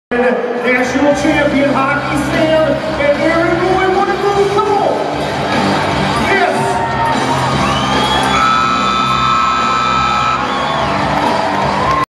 Aaron Roy rides Honky Sam for 84.5 points (PBR)
at the 18th annual Coronation Royal Rumbull PBR at the Coronation Arena in Coronation, Alberta on August 15th, 2018.

Sanctioned by the PBR Canada Touring Pro Division.